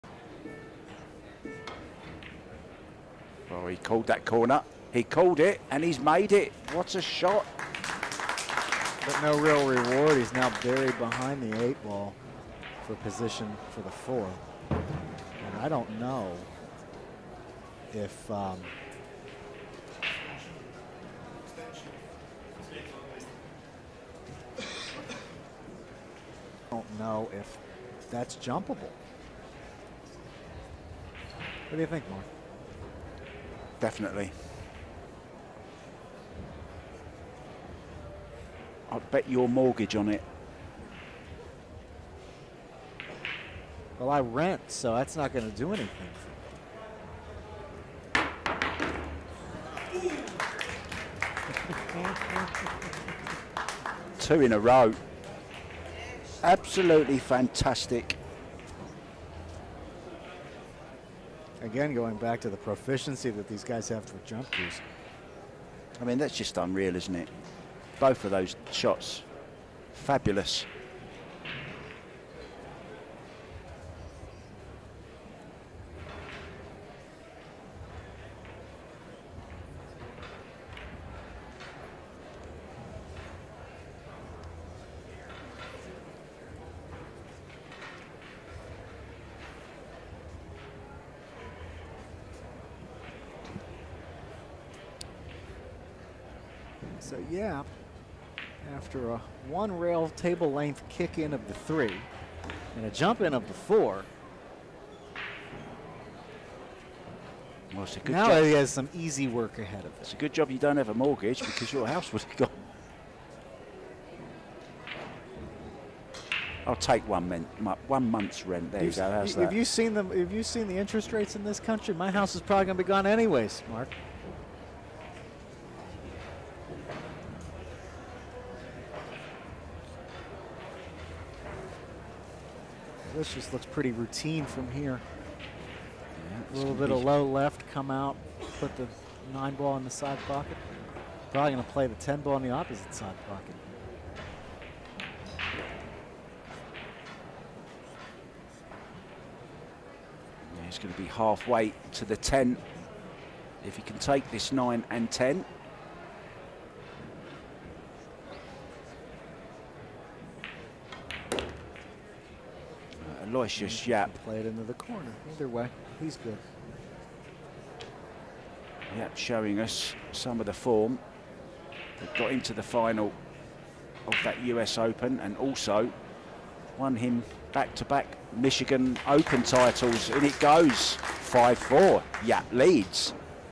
YAPP is on beast mode! Crazy 10-Ball runout
▸ 🇪🇸 FSR vs 🇸🇬 Aloysius Yapp - Quarterfinals
▸ Predator WPA Men’s World 10-Ball Championship 2023